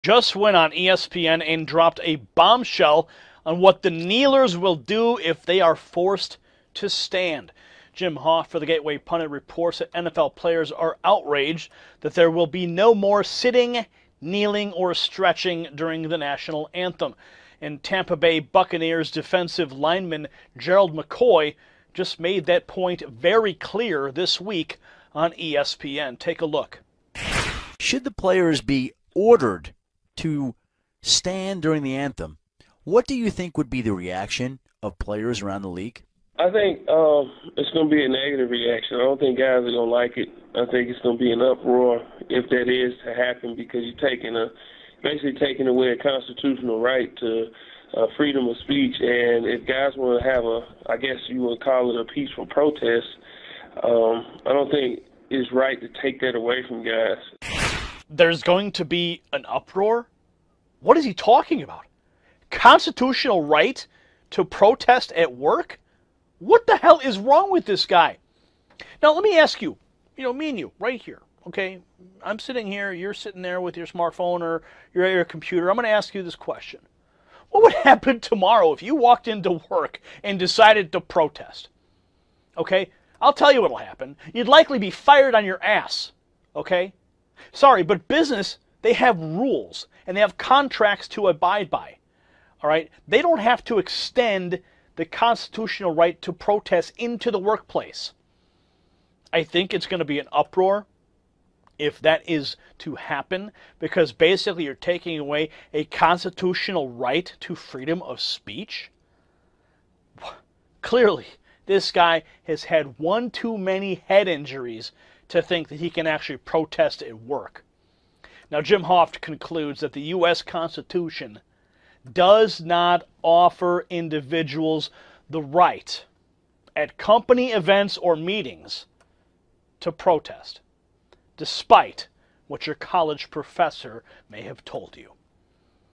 NFL LINEMAN JUST WENT ON ESPN AND DROPPED A BOMBSHELL ON WHAT KNEELERS WILL DO IF FORCED TO STAND
SOURCE:
NFL Lineman Warns “It’s Gonna Be an Uproar!” If Players Are Forced to Stand for the National Anthem
NFL players are outraged that there will be no more sitting, kneeling or stretching during the national anthem.Tampa Bay Buccaneers defensive lineman Gerald McCoy says there’s “gonna be an uproar” if players are told to stand during the national anthem like they did for the last 50 years.

“I think it’s gonna be an uproar if that is to happen because you’re basically taking away a constitutional right to freedom of speech.”